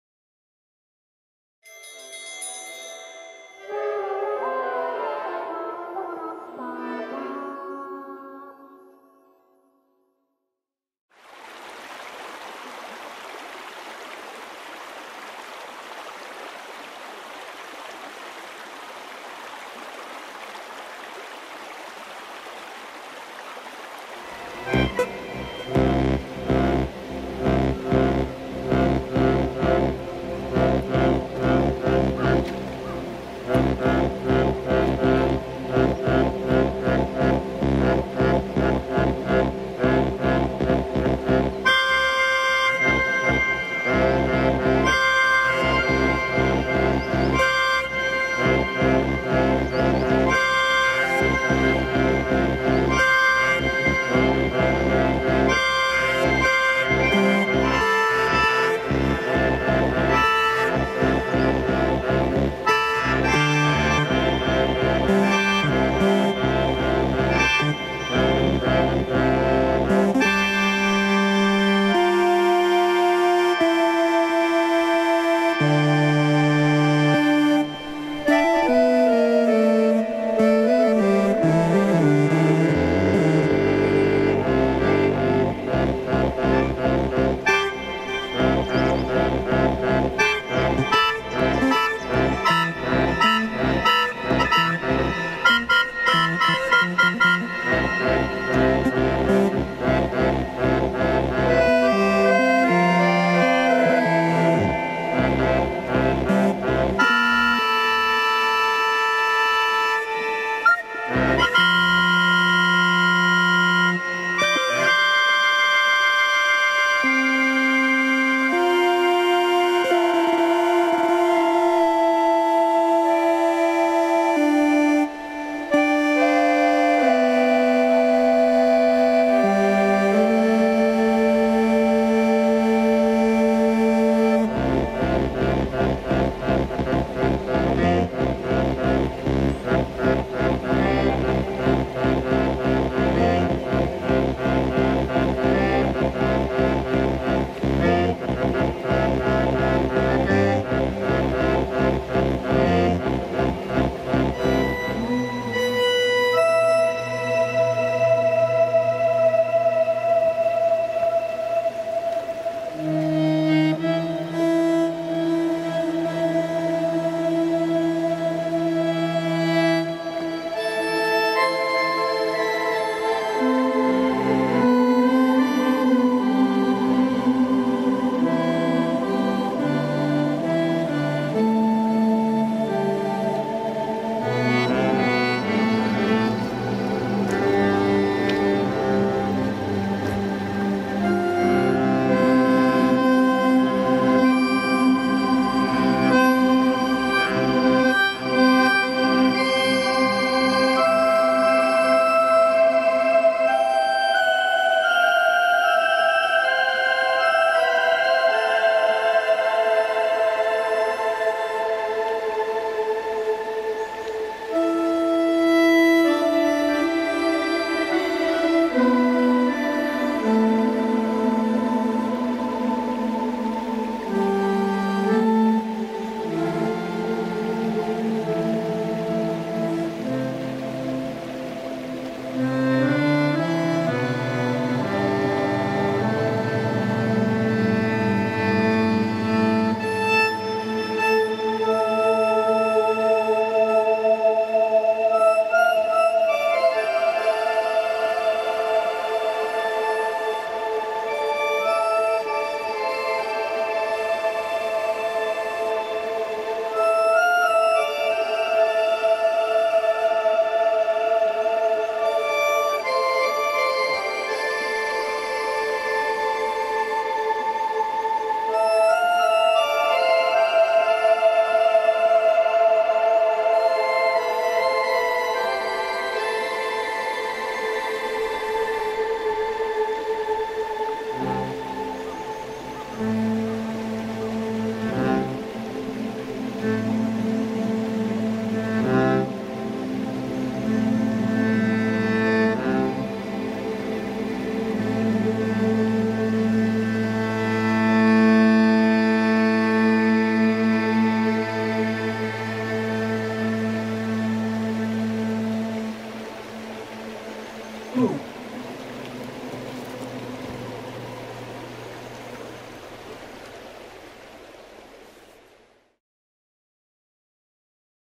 野外即興 20160330 朽木渓谷 高島市
2016/03/30 野外即興「Meeting of the Hearts」
滋賀県高島市朽木渓谷にて収録。

JVC GC-XA1
Zoom H6
IK Multimedia iLoud
Hammond 44 PRO
MFB Nanozwerg
Zoom MS-70CDR

■演奏・収録・編集 山口ミチオ

(c)Michio Yamaguchi (Project Meeting of the Hearts)